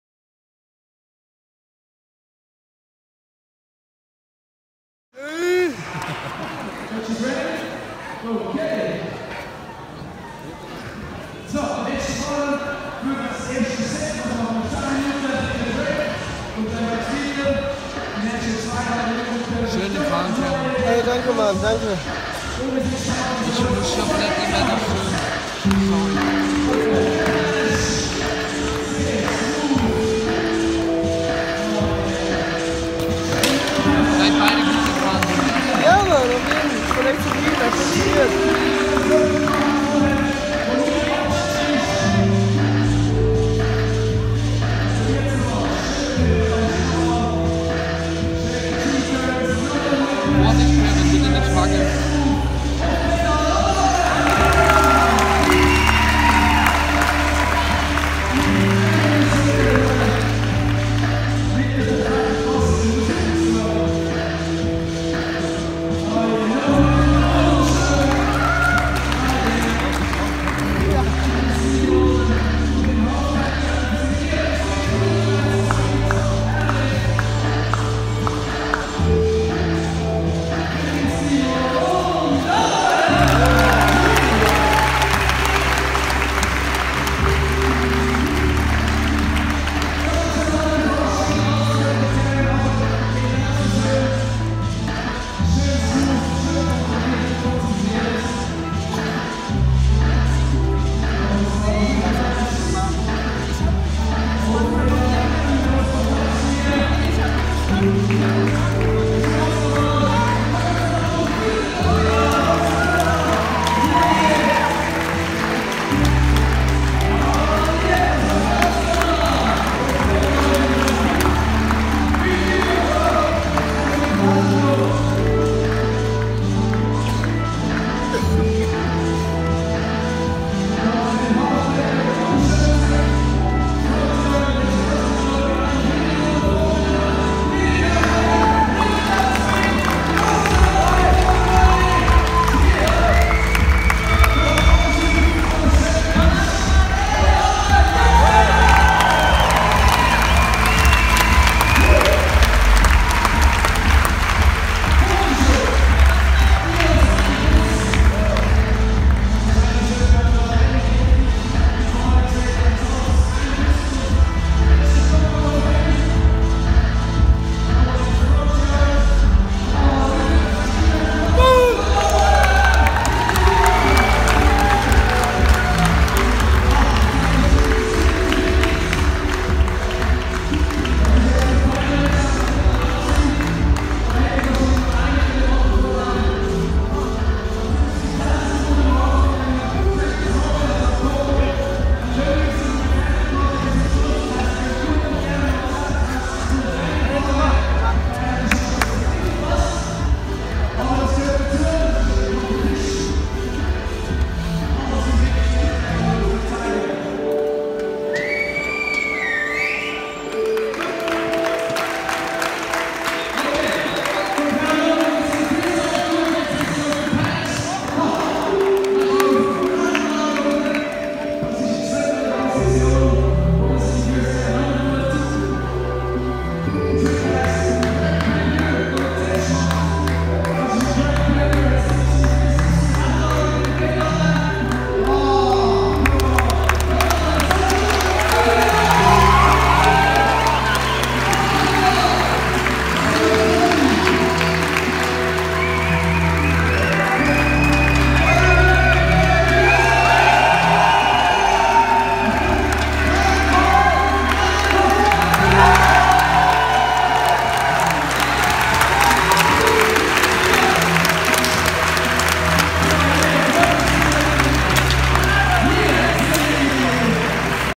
Thomas Noyer - 4th Bike Days 2017
Thomas Noyer finished up in 4th place at the 2107 Bike Days contest in Solothurn, Switzerland. Check out his run plus the bonus round which helped bump up his overall score.
Thanks to Dan Hennig for filming.